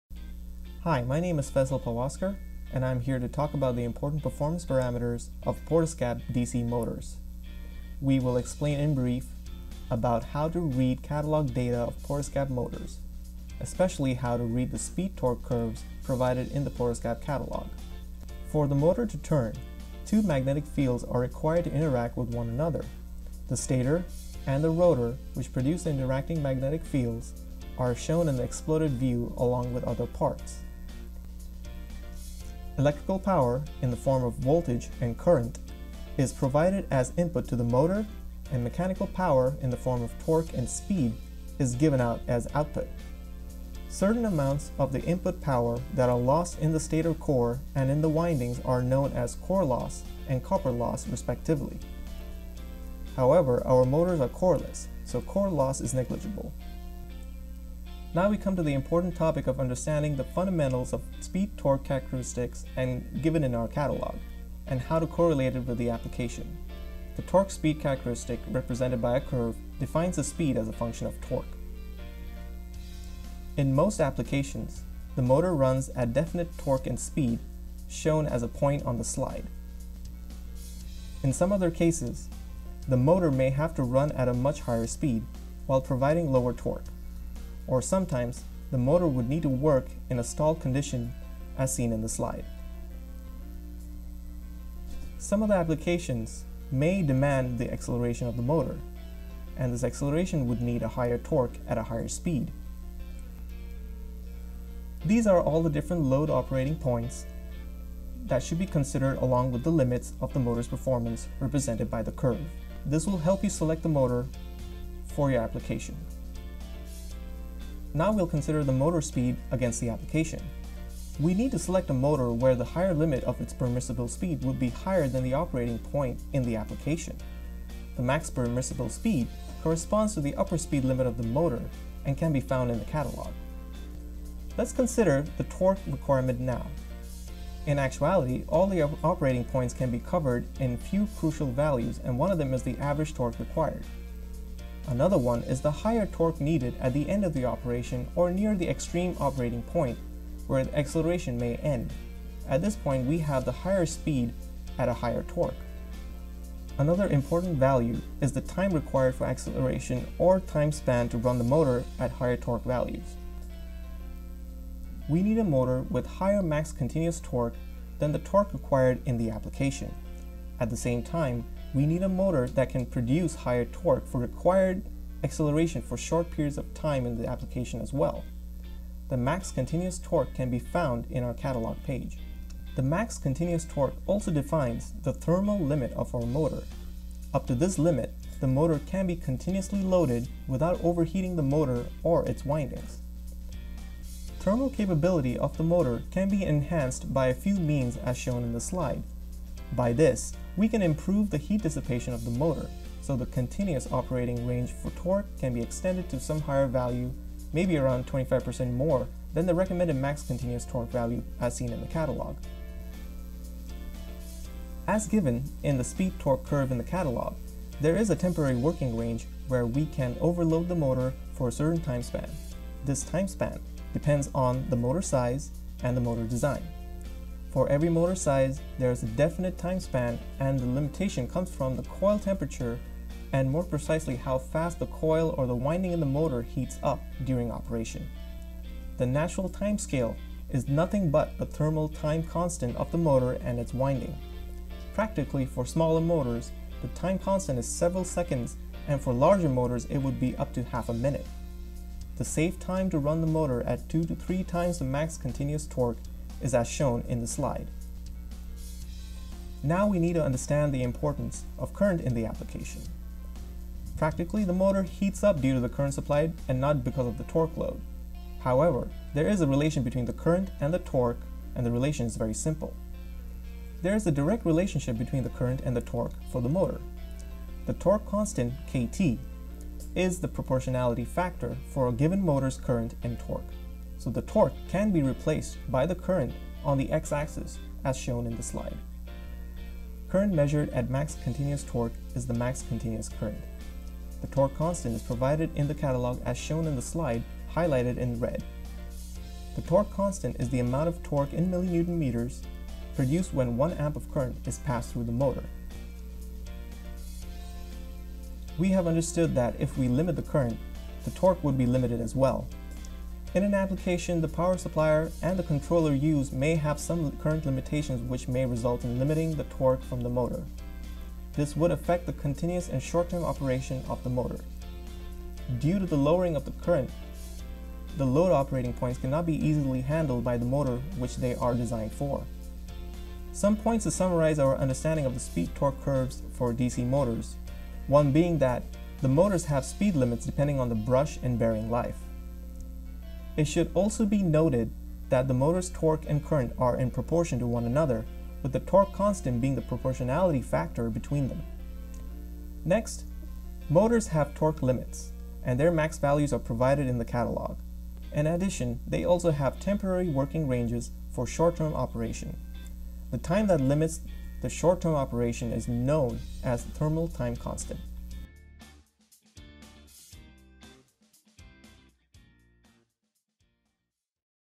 Speed - Torque Characteristics of DC Motors by Portescap
Learn about the speed-torque characteristics of DC Motors from Portescap Application Engineer, Faisal Pawaskar.